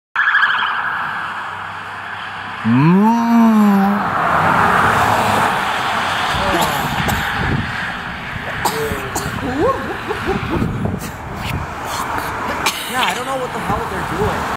After CAEH OCCUPATION Smoke Break
Cops Went Speeding By! Scared me, road being so close.
"Happens all the time." said a local.